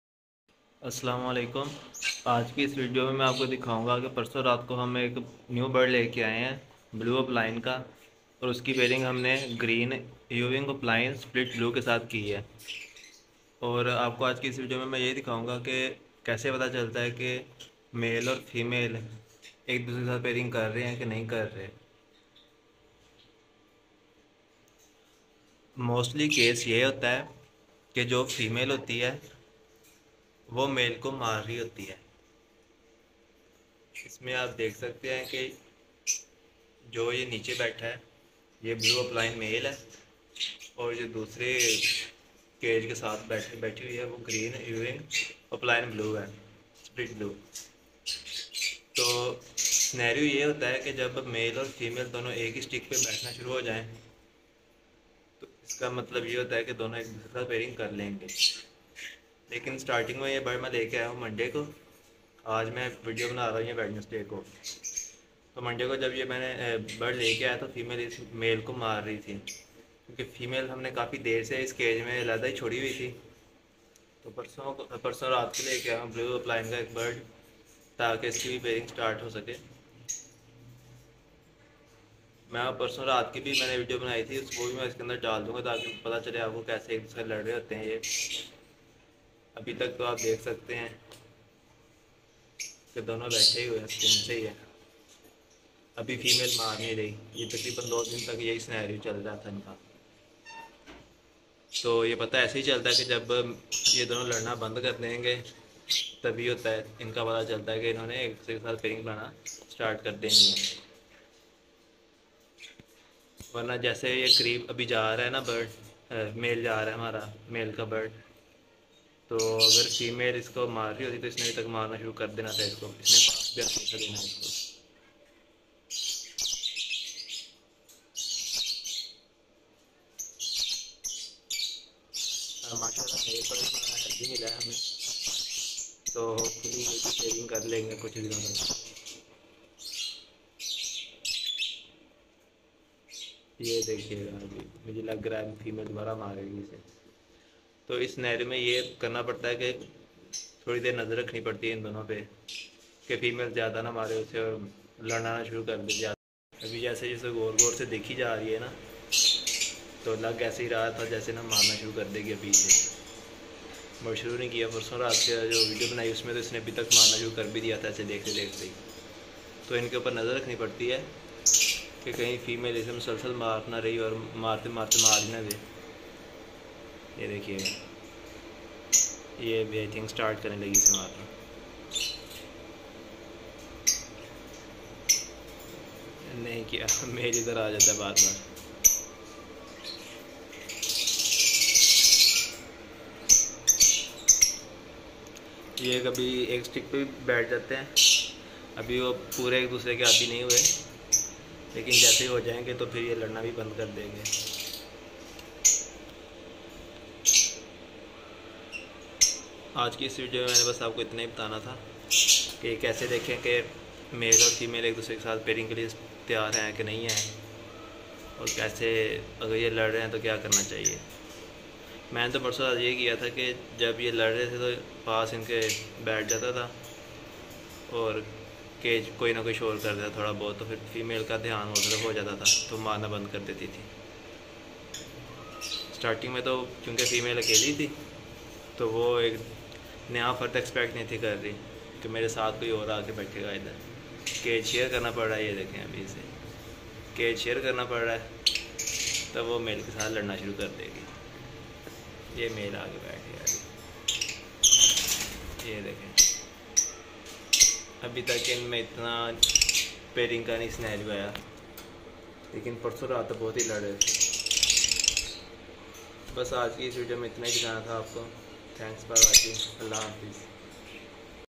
Naye love bird sa purany love bird ke laria shuru | Daikhy iska hal | Shahid Bashir Aviary
Naye love bird sa purany love bird ke laria shuru Daikhy iska hal